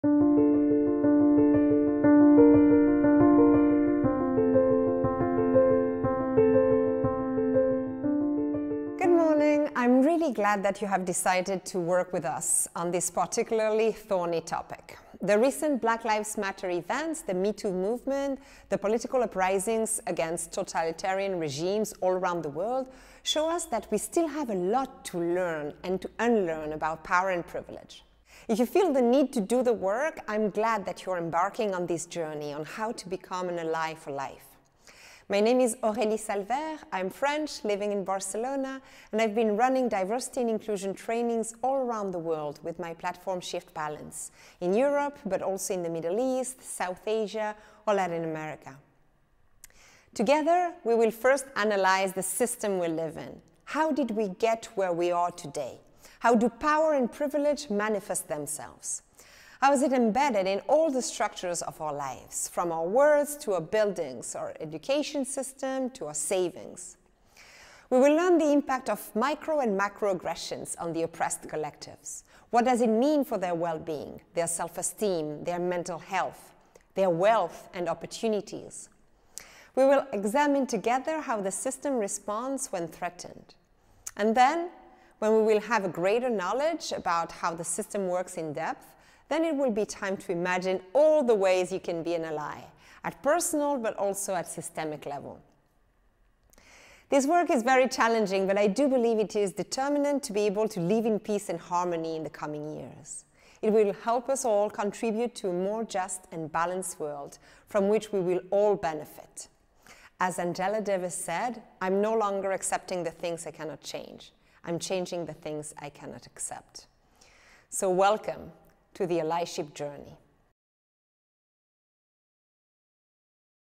Introduction to the Allyship Journey
Good morning! I am really glad that you have decided to work with us on this particularly thorny topic.
The recent Black Lives Matter events, the metoo movement, the political uprisings against totalitarian regimes all around the world show us that we still have a lot to learn and to unlearn about power and privilege.
If you feel the need to do the work, I am glad that you are embarking on this journey on how to become an ally for life.
My name is Aurélie Salvaire, I am French, living in Barcelona. I have been running diversity and inclusion trainings all around the world with my platform Shiftbalance, in Europe but also in the Middle East, South Asia or Latin America.
Together, we will first analyse the system we live in. How did we get where we are today? How do power and privilege manifest themselves? How it is embedded in all the structures of our lives, from our words to our buildings, from our education system to our savings.
We will learn the impact of micro and macro aggressions on the oppressed collectives. What does it mean for their well-being? Their self-esteem? Their mental health? Their wealth and opportunities? We will examine together how the system responds when threatened.
And then, when we will have a greater knowledge about how the system works in depth, then it will be time to imagine all the ways you can be an ally, at personal but also at systemic level.
This work is very challenging, but I do believe it is determinant to be able to live in peace and harmony in the coming years. It will help us all contribute to a more just and balanced world from which we will all benefit.
As Angela Davis said, “I am no longer accepting the things I cannot change. I am changing the things I cannot accept.”

So welcome to the Allyship journey!